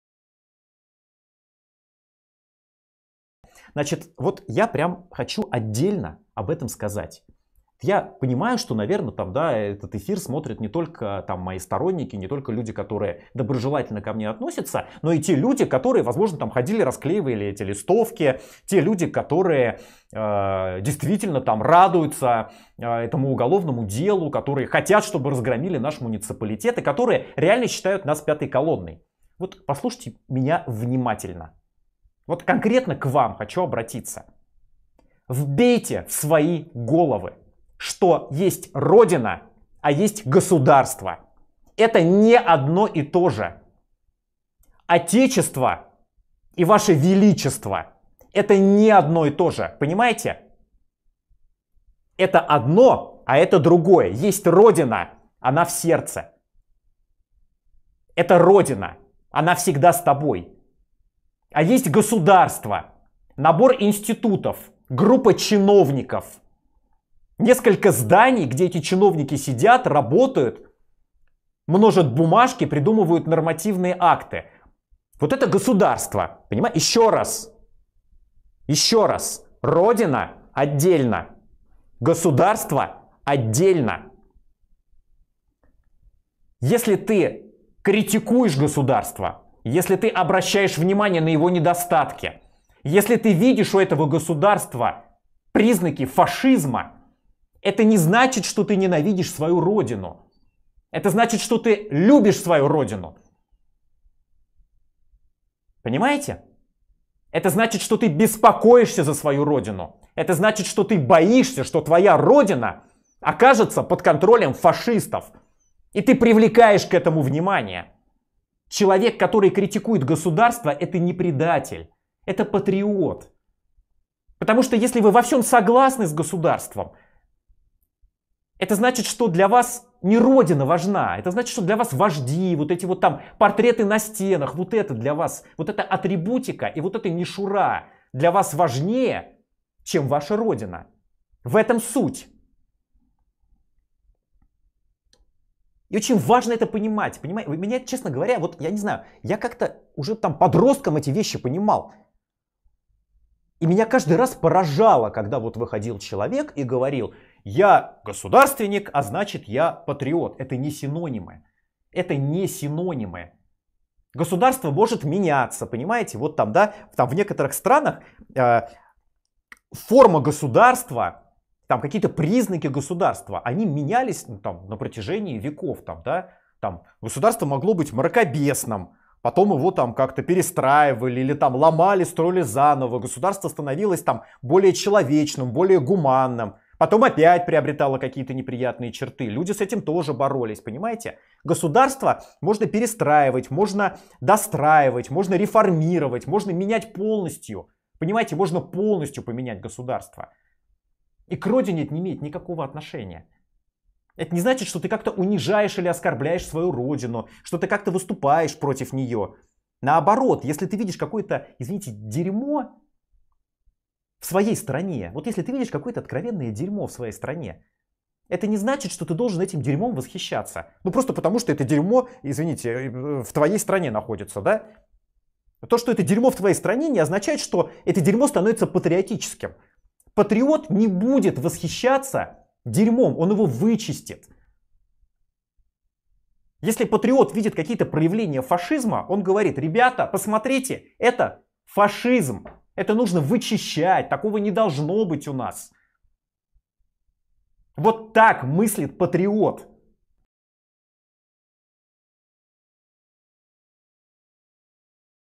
Ilja Jašin o lažnom patriotizmu
Ovo je Ilja Jašin, poslanik Moskovske opštine Krasnoseljski i jedan od poslednjih ruskih opozicionara u životu i na slobodi u Rusiji, i jedan od najhrabrijih ljudi za koga znam.

Ovde objašnjava razliku između "otadžbine" i "države", a takođe šta je pravi patriotizam (sa srpskim titlovima).